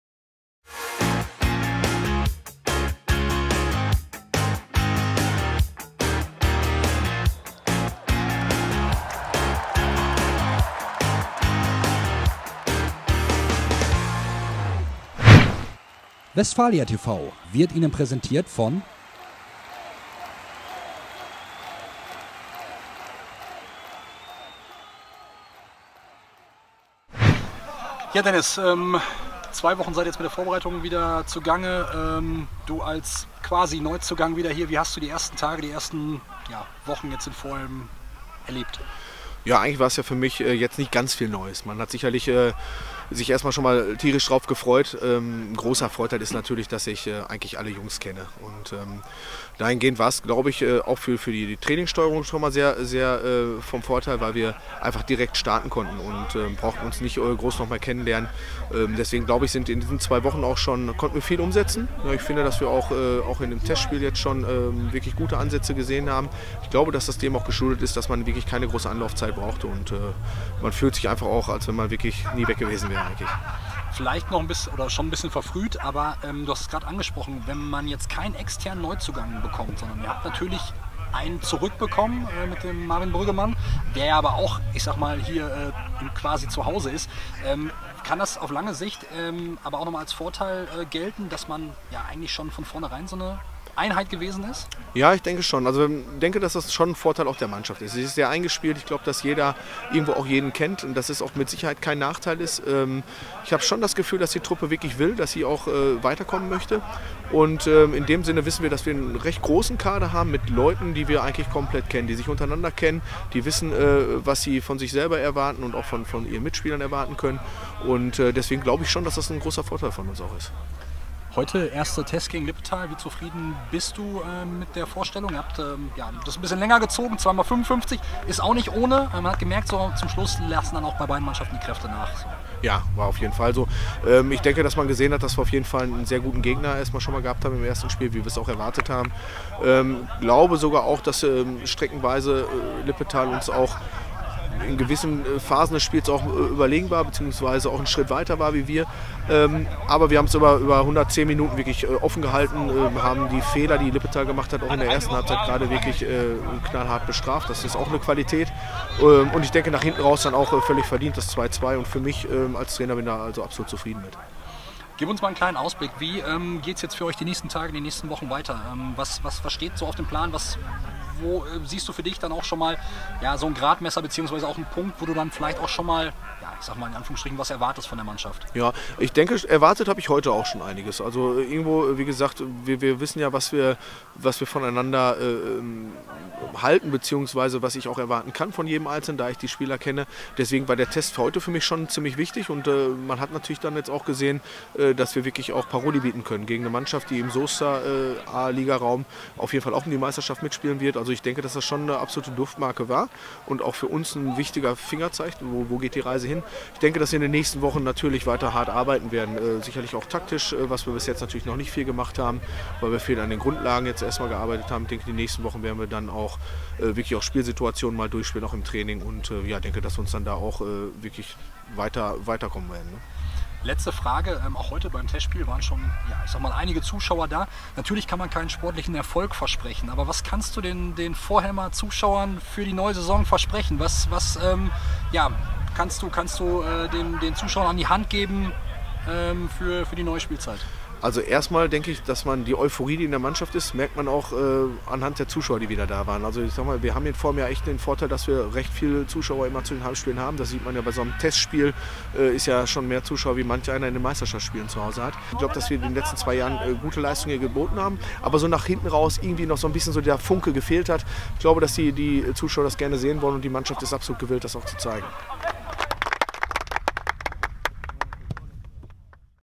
Interview und erste Eindrücke nach unserem ersten Testspiel
Interview mit unserem Trainer Dennis Averhage nach unserem ersten Test gegen Lippetal